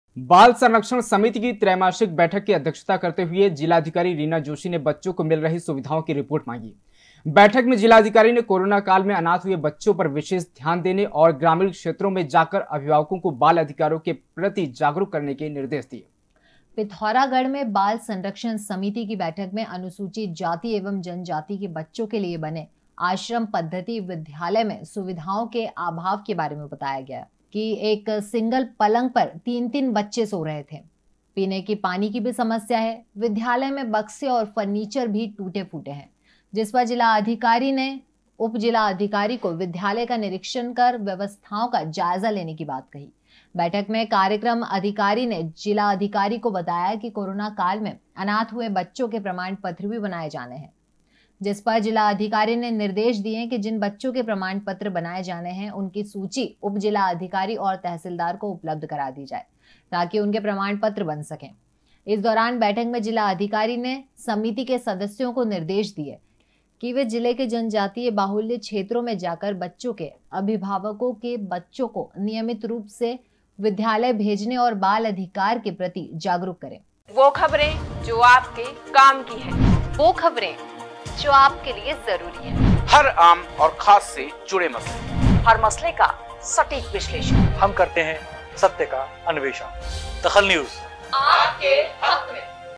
DAKHAL NEWS बाल संरक्षण समिति की हुई त्रैमासिक बैठक
बाल संरक्षण समिति की हुई त्रैमासिक बैठक
जिलाधिकारी रीना जोशी ने की बैठक की अध्यक्षता